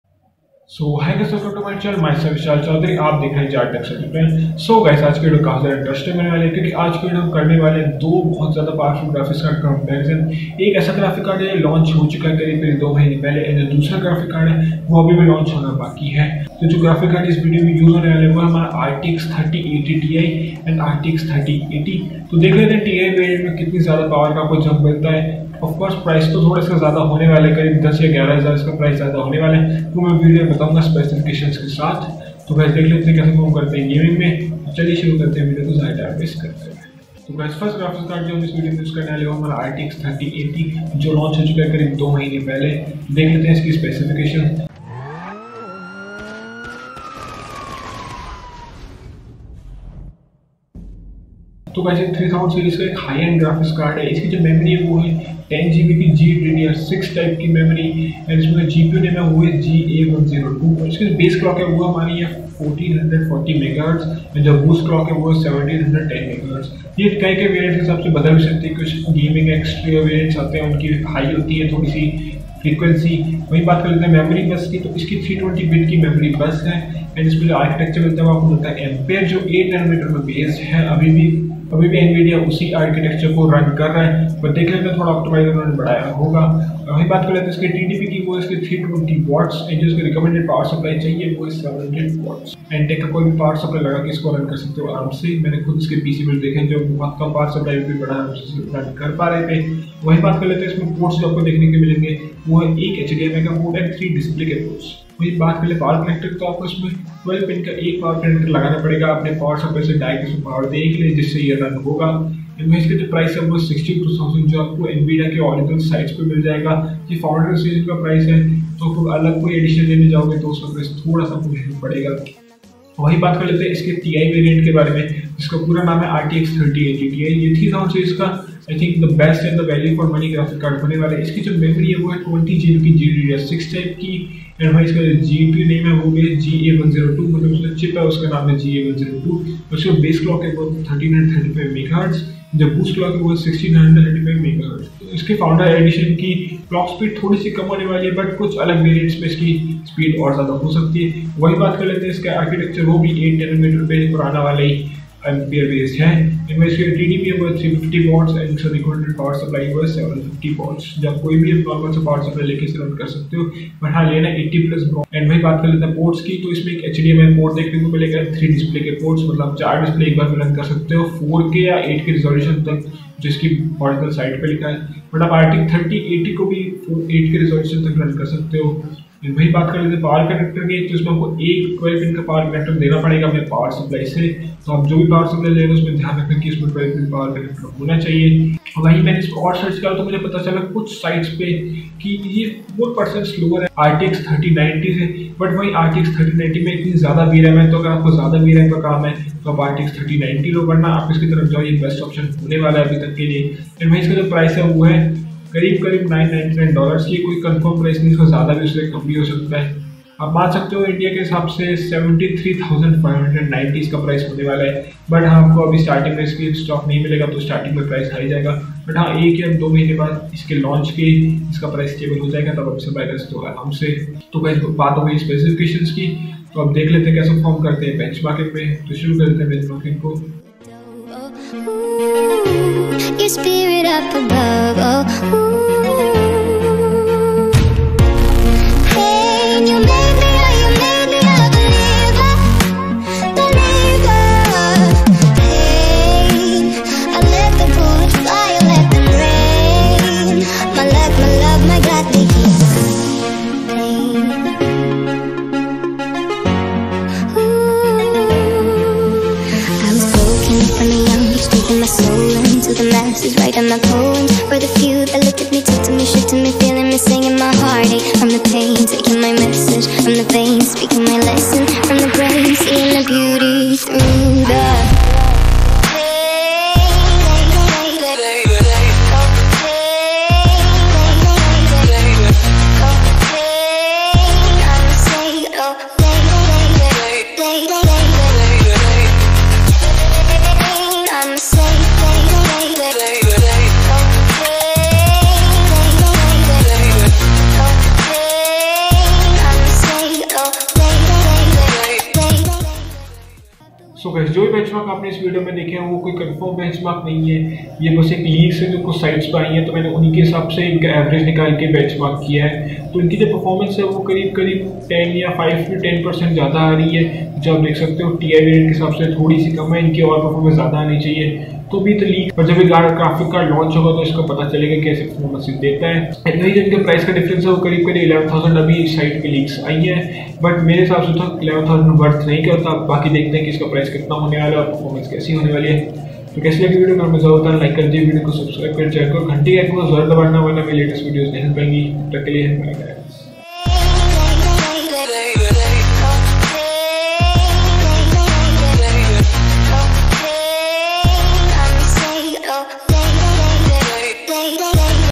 RTX 3080 Ti vs RTX 3080 || AMD vs NVIDIA || Test In 5 Games
in this video - we are going to compare rtx 3080 ti vs rtx 3080

specification-
rtx 3080 ti
series 3000
memory - 20gb gddr6x
gpu name - ga102
bas clock - 1335 mhz
architechture - ampere 8nm based
tdp - 350 w
recommended psu - 750 w
ports - 1 hdmi and 3 display ports
power connector - 1*12 pin
according to sites - 4% slower than rtx 3090 which is 24 gb

rtx 3080
series 3000
memory - ggdr6x
gpu name - ga102
architechture - ampere 8nm
tdp - 320 w
recommended psu - 700 w
ports - 1 hdmi  adn 3 display
ppowerconnector - 1*12 pin
price - 62000 for founders eddition

Jaat Techs